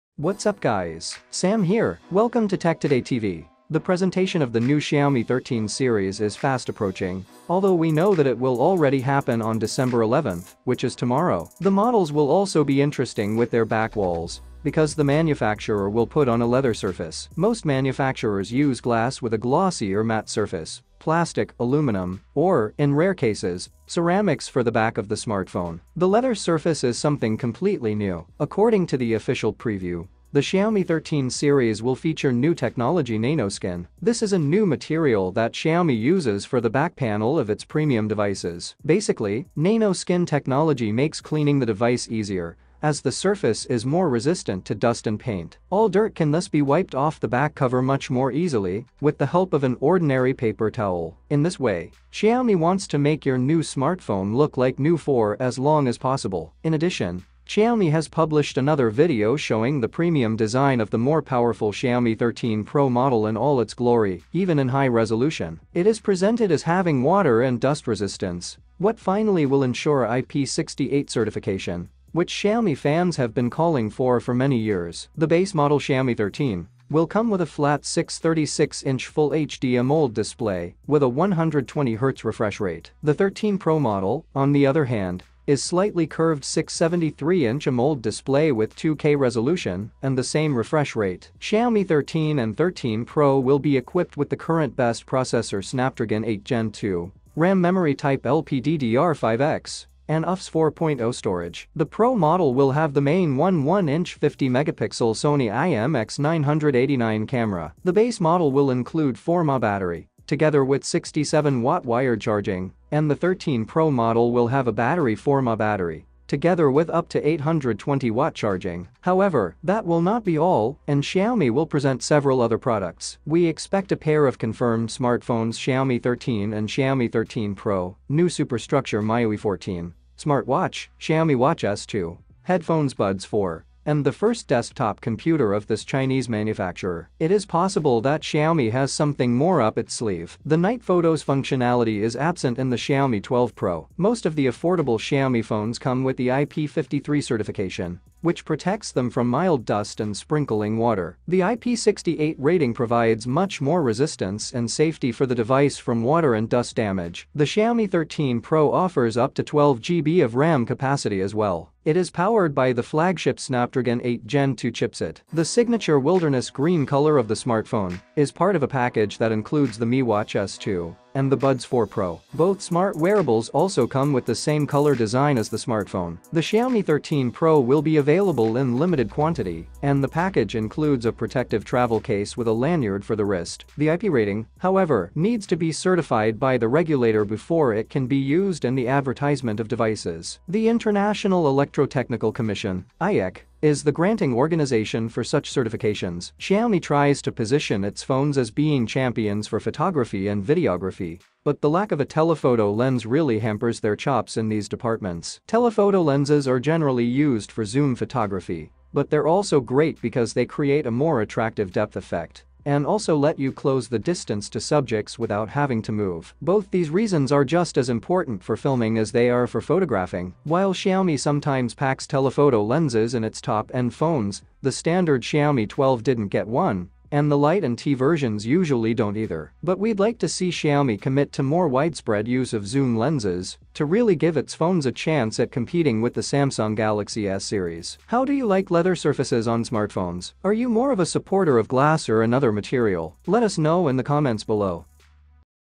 Xiaomi 13 Pro - THANKS XIAOMI!
The Xiaomi 13 Pro will be available on Tomorrow, December 11. It will feature a new camera design and a fancy green hue, but more significantly, it will finally receive an official IP classification. In a flashy video shared on Weibo, the business claimed that the phone will be dust and water resistant.

The camera will be able to snap shots in any environment, including a mode for photographing stars at night, which the Xiaomi 12 Pro lacks.